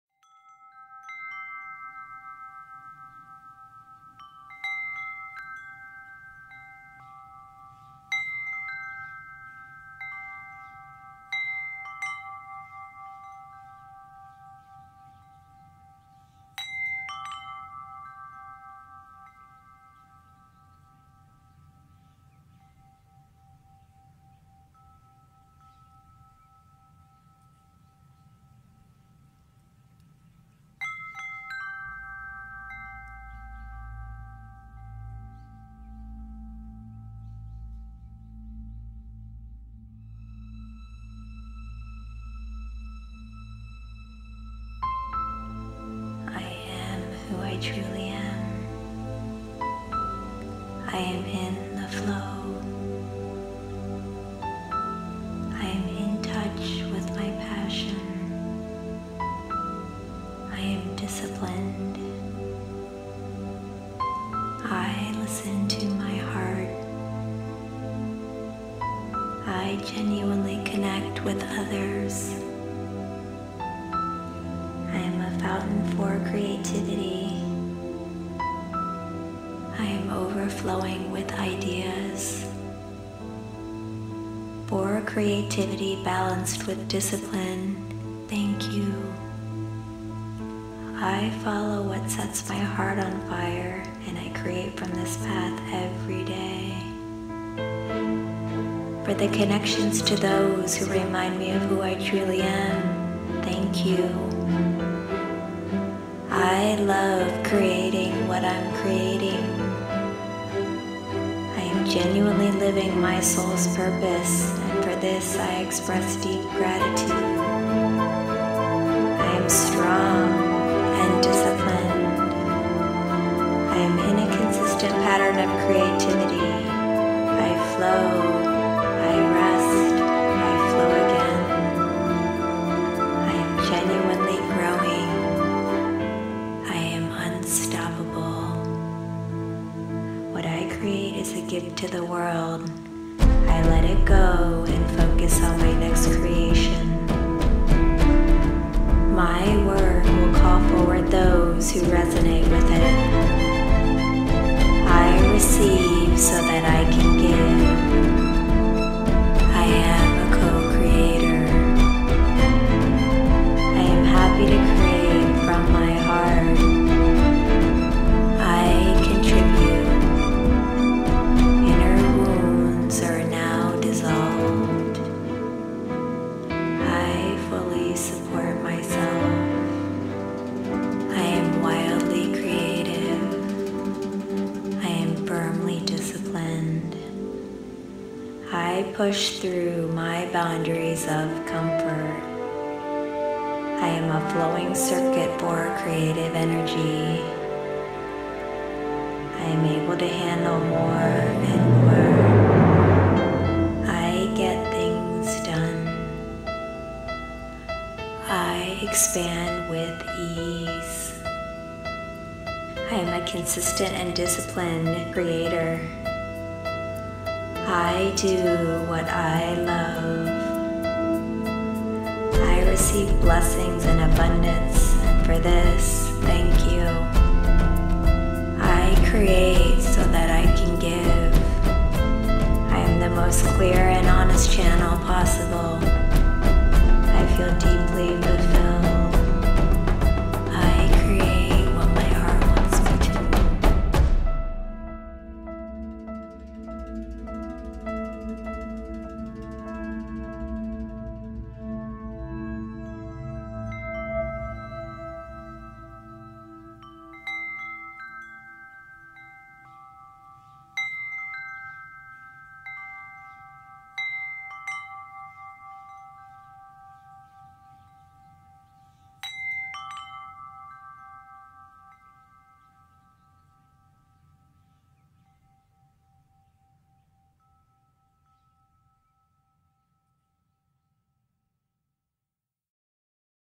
Cultivating Creativity & Discipline Affirmations
To express our full range of creativity, we must be disciplined in our actions.  Otherwise, our ideas just hang out in the ether and never become physicalized.